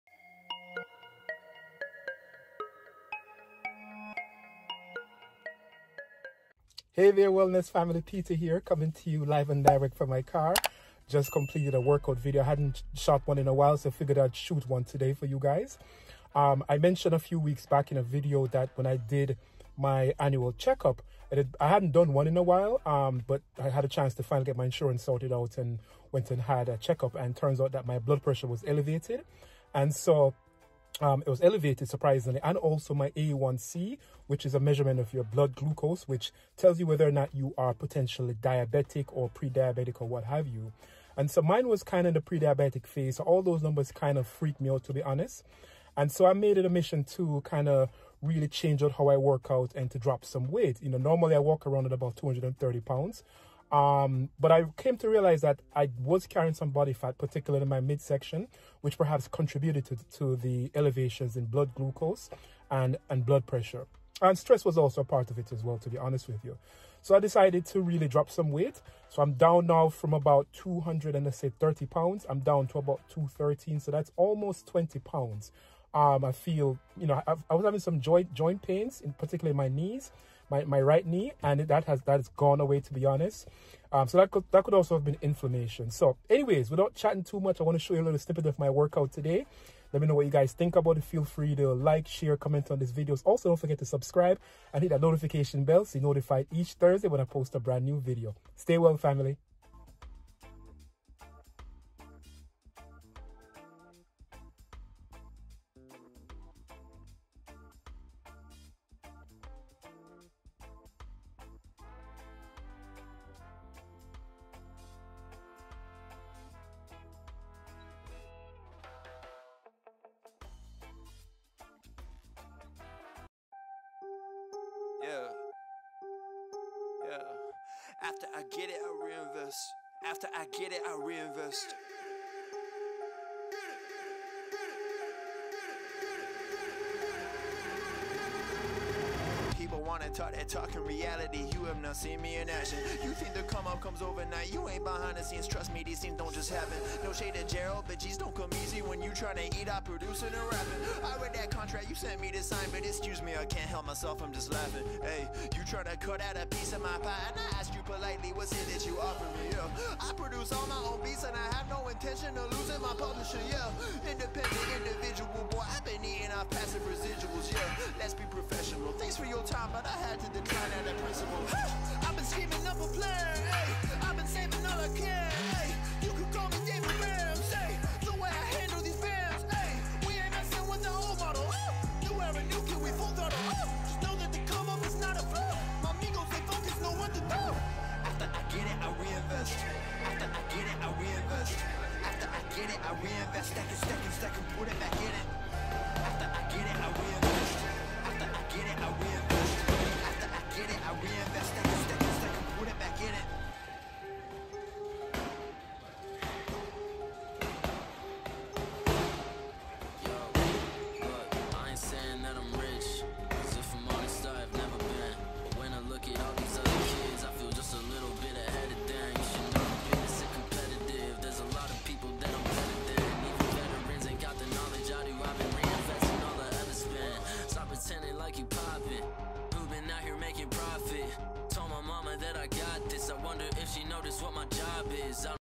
New Year Strength And Conditioning Workout
Here is a snippet of my first strength and conditioning workout of 2021. I recently decided I needed to lose weight as a result of my last doctors visit, where it was revealed that my blood pressure was elevated, as well as my hemoglobin a1c (blood glucose).

I am currently down about 18lbs after just under a month of dieting, and changing my approach to working out. My aim of course is to bring my blood pressure and hemoglobin a1c levels down through continued lifestyle adjustments versus taking medication. I have also incorporated some deep belly breathing, and other mindfulness approaches to lower stress levels and improve mental health. As a reminder you can't have mental health without physical health and vice versa. As always stay well family.